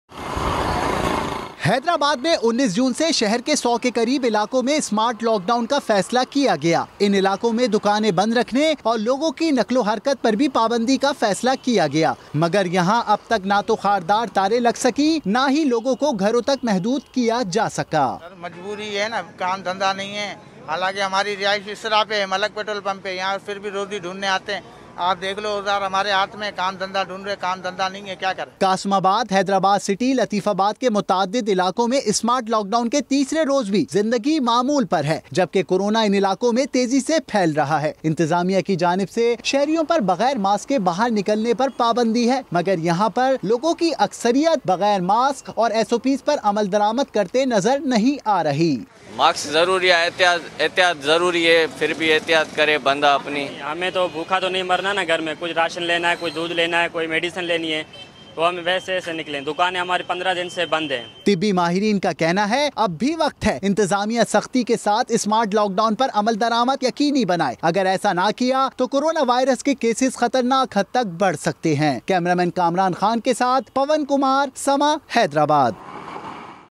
Hyderabad, smart lockdown is not implemented, people are not taking precautions | SAMAA TV
Hyderabad: Coronavirus is spreading rapidly in Hyderabad, therefore govt implemented a smart lockdown on June 19 but no necessary measures are taken by the authorities and people. Watch this report by Pawan Kumar.